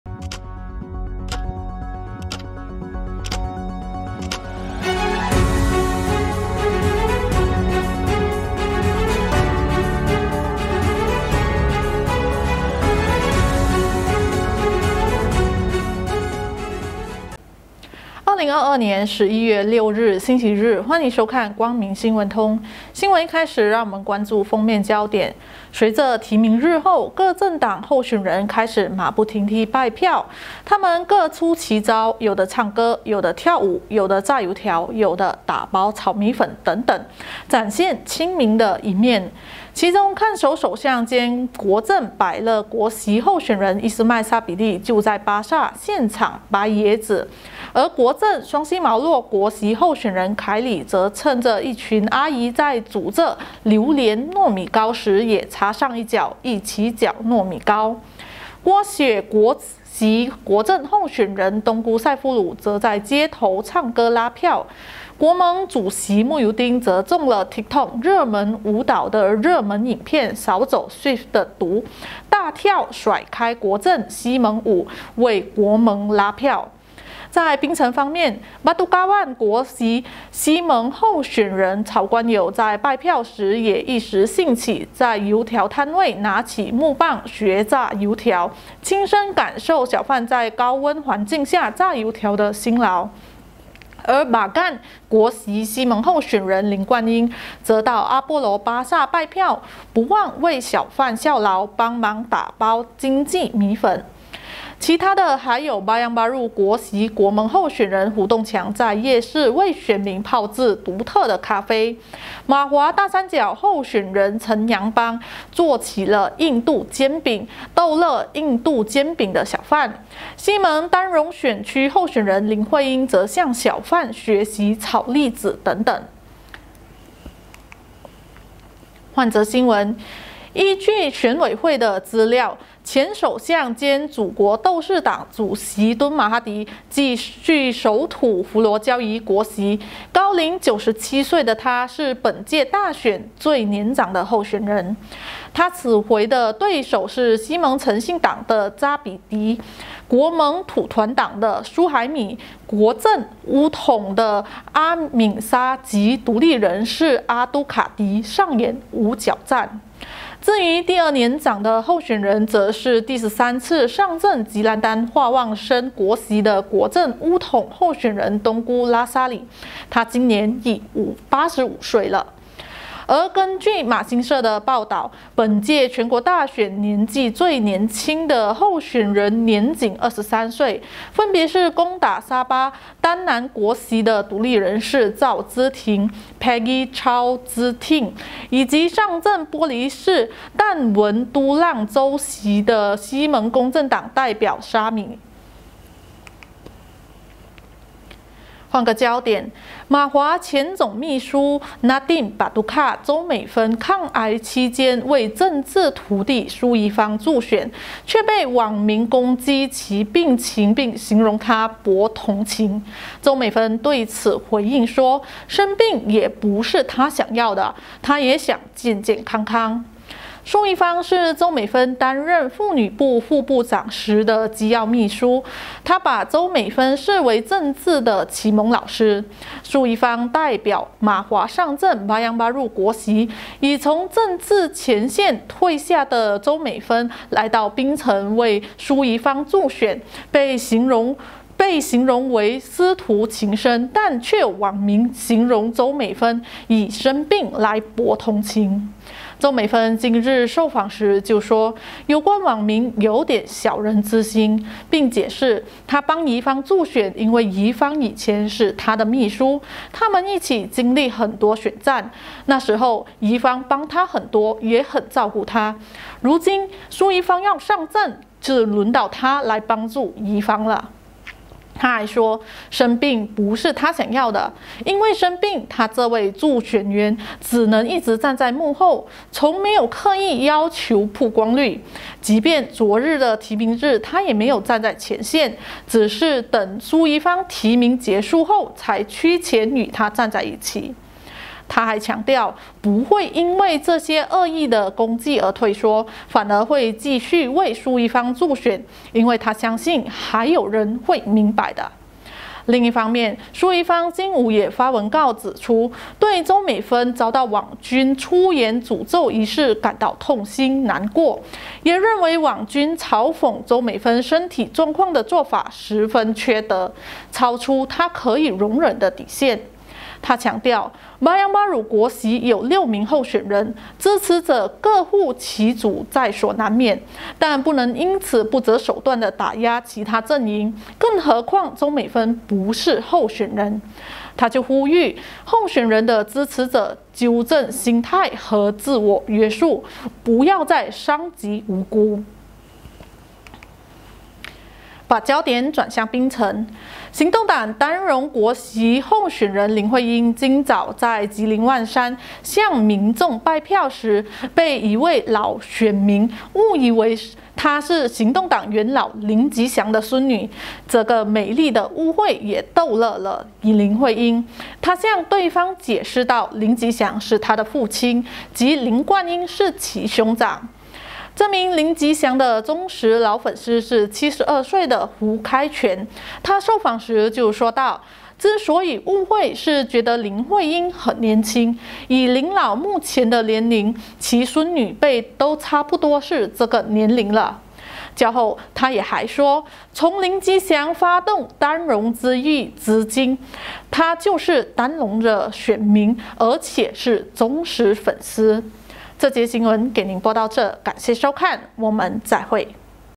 【光明新聞通】2022年11月7日夜報封面焦點
◆各路候選人拜票
◆97歲敦馬最老候選人 最年輕候選人僅23歲
◆帶病為蘇儀芳站台被抨 周美芬稱非博同情
◆被誤認為林吉祥孫女 逗樂林慧英連忙解釋

光明新聞通，讓您掌握新聞時事，一點就通！與你一同關注7/11/2022光明夜報焦點。欲知詳細新聞內容，購閱《光明日報》，點擊 或訂閱光明電子報